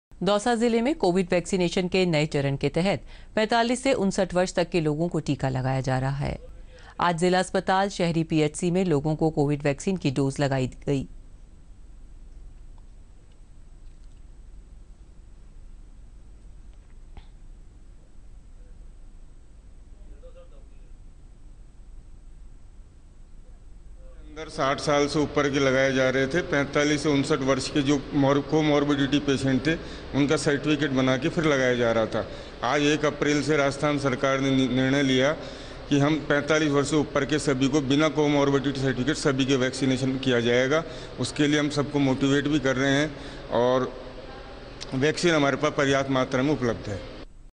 वैक्सीनेशन तीसरा चरण के तहत 45 से 59 वर्ष तक के लोगों को टीका लगाया जा रहा है : दौसा
दौसा जिले में कोविड वैक्सीनेशन के नए चरण के तहत 45 से 59 वर्ष तक के लोगों को टीका लगाया जा रहा है। आज जिला अस्पताल, शहरी पीएचसी में लोगों को कोविड वैक्सीन की डोज लगाई जा रही है।